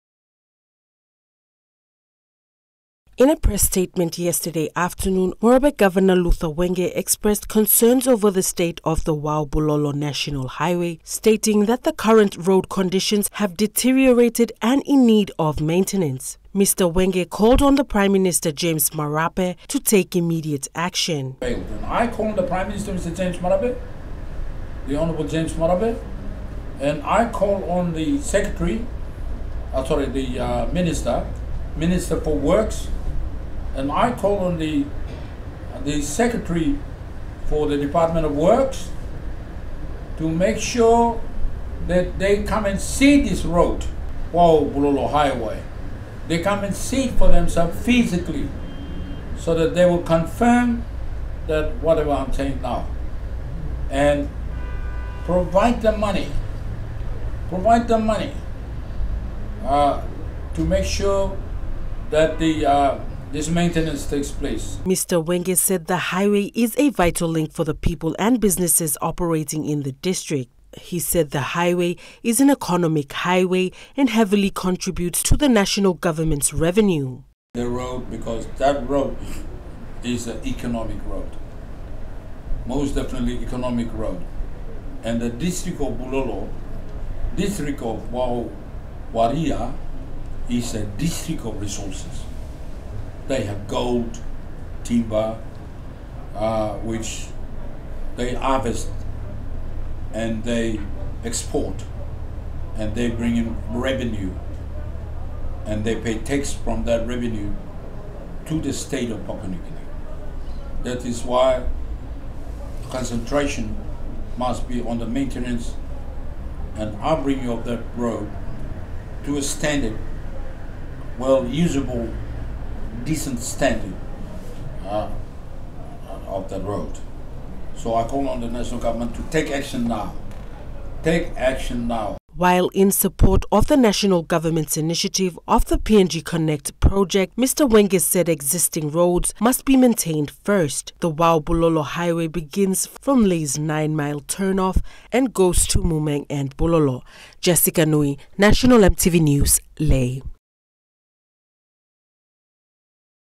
WENGE ON WAU BULOLO HIGHWAY
Morobe Governor Luther Wenge has raised concerns over the current state of the Wau-Bulolo National highway and called on the national government to take immediate action.